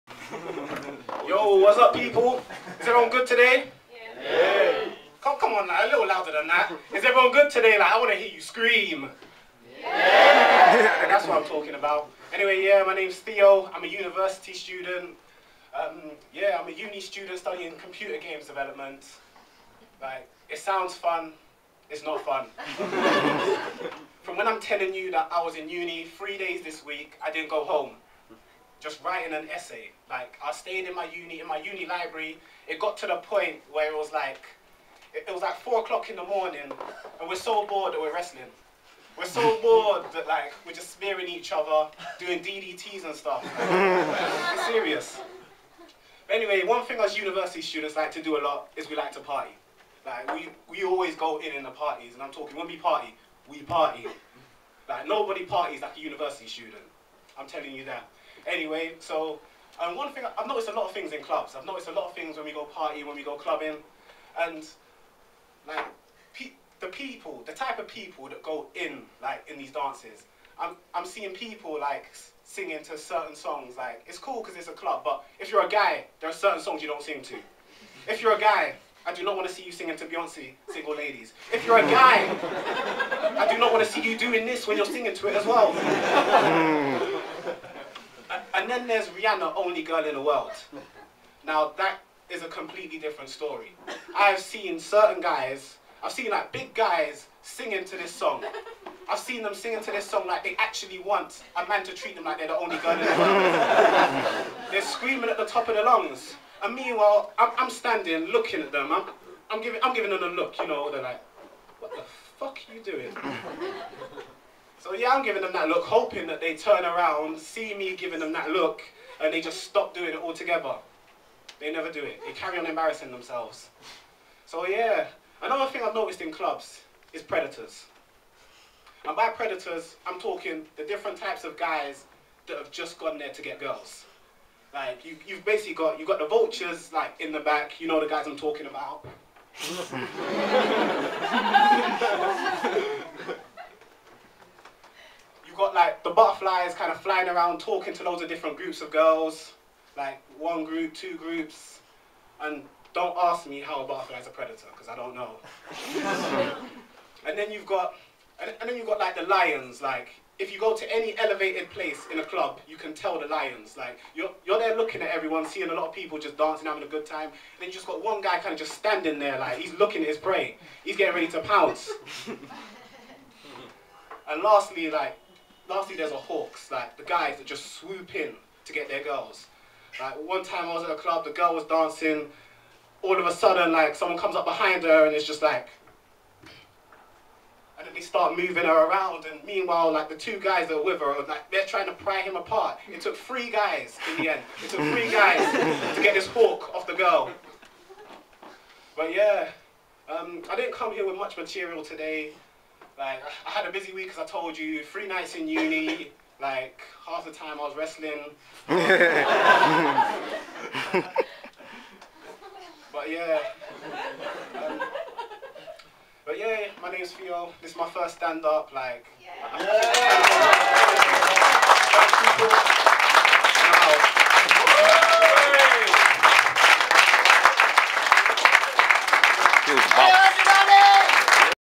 Theo Ajiey - 1st Stand Up At the Camden Head
Theo Ajibode does his 1st ever stand up at the Camden Head in Angel, Islington, London